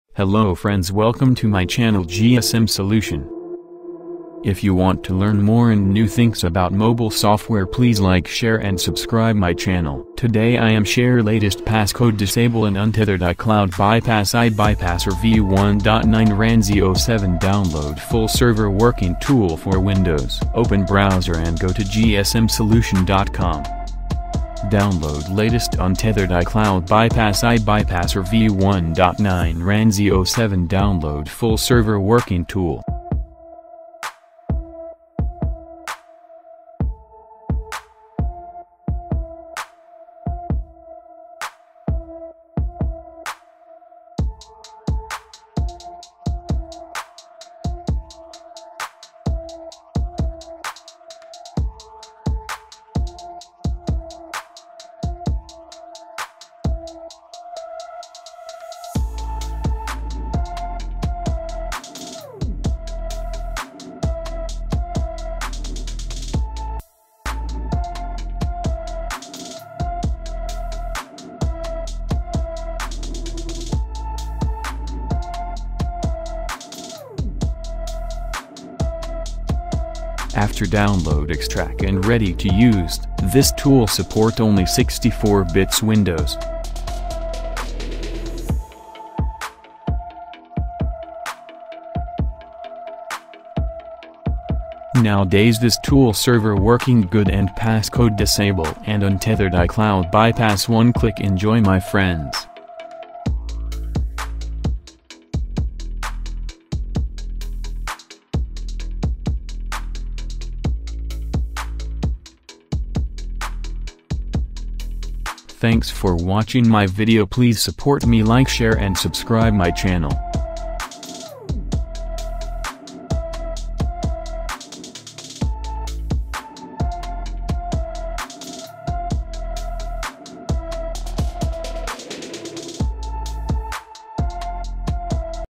[Windows]Latest Passcode-Disable & Untethered iCloud Bypass iBypasser v1 9 Ranzhie07 Download.
Note:Now adays many tool server down and not working but this working now good and this support only windows 64bit only.
2# See step by step in video, if you have error in process bypass icloud. pls comment here, i will reply soon.